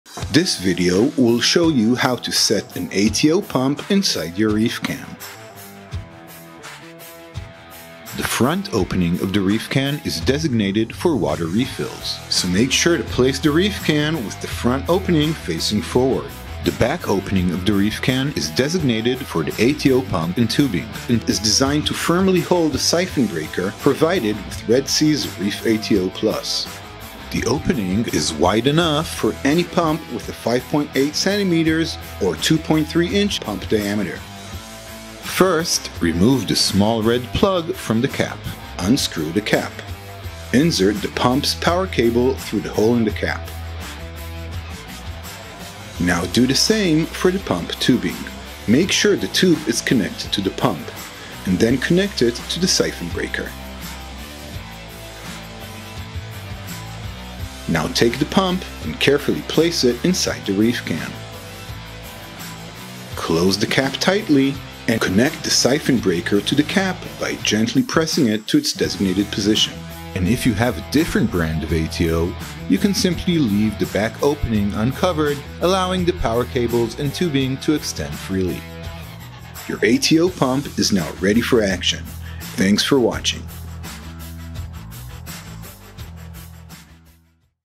How to set an ATO pump inside your ReefCan
This video will show you how to set an ATO pump inside your ReefCan.
The back opening of the ReefCan is designated for the ATO pump and tubing. It is wide enough for any pump with a 5.8cm or 2.3” pump diameter, and its cap is designed to firmly hold the siphon breaker provided with Red Sea’s ReefATO+.

We are constantly making videos designed to empower you with knowledge.